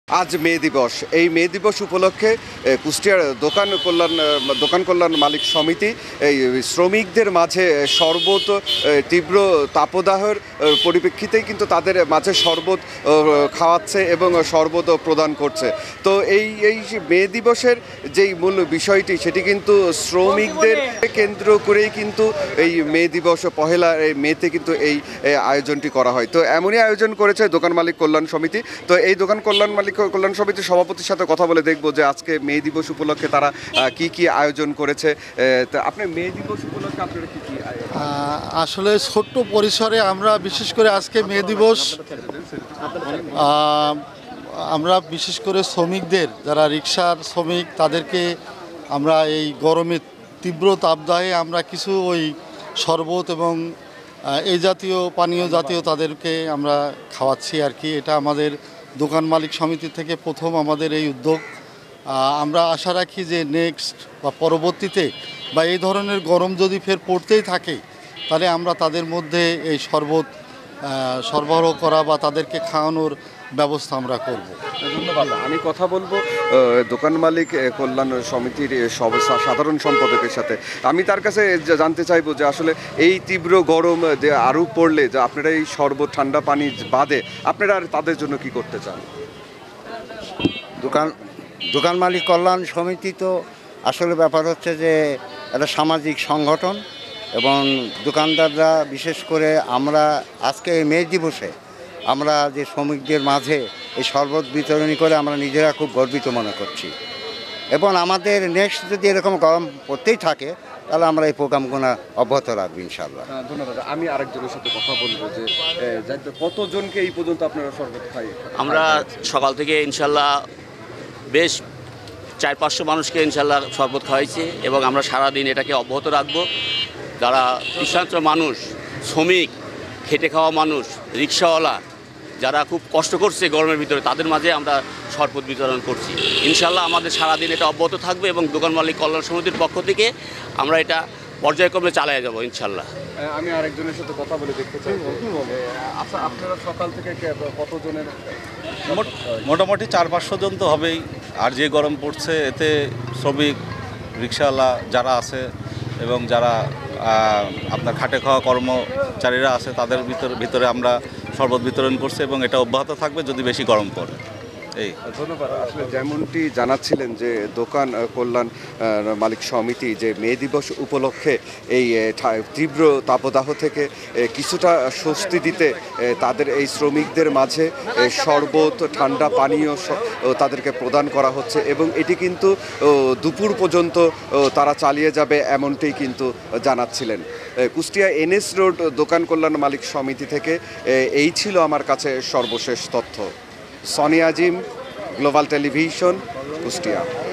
কুষ্টিয়ায় মে দিবস উপলক্ষে শ্রমিকদের মাঝে শরবত বিতরণ করেছেন দোকান কল্যাণ মালিক সমিতি | Global TV
কুষ্টিয়ায় মে দিবস উপলক্ষে শ্রমিকদের মাঝে শরবত বিতরণ করেছেন দোকান কল্যাণ মালিক সমিতি | Global TV News